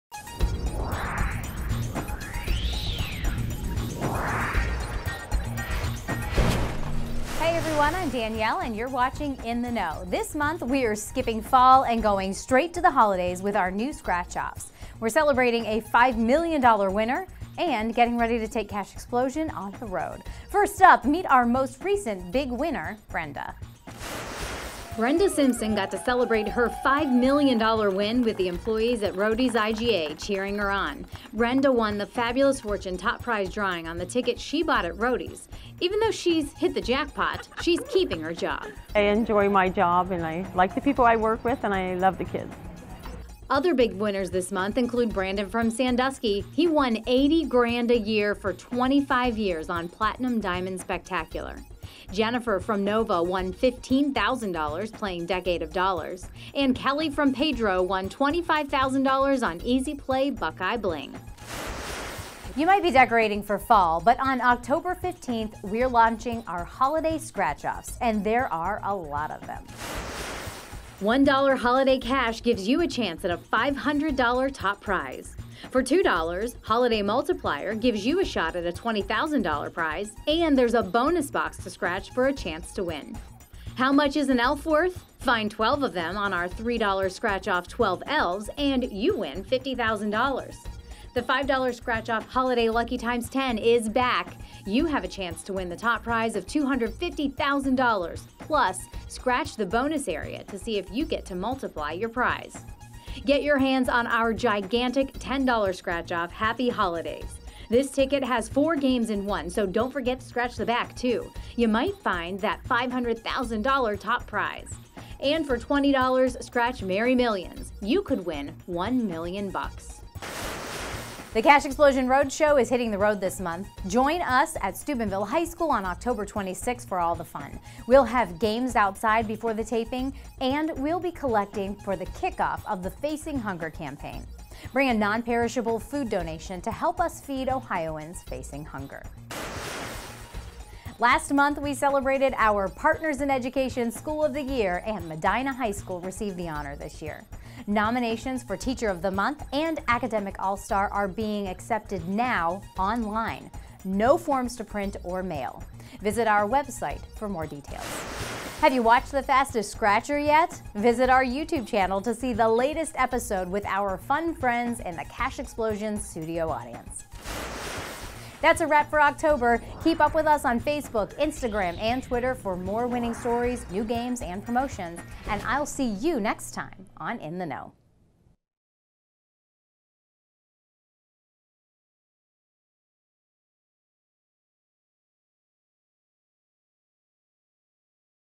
In the Know | October 2019
It may be October but it's never too early for Holiday Scratch Offs! This month we've got six new scratch offs and a $5 million winner! And don't miss the details on the Cash Explosion Roadshow!

Big Winners: 0:23
New Scratch Offs: 01:02
Cash Explosion Roadshow: 02:00
School of the Year: 02:20
Fastest Scratcher: 02:38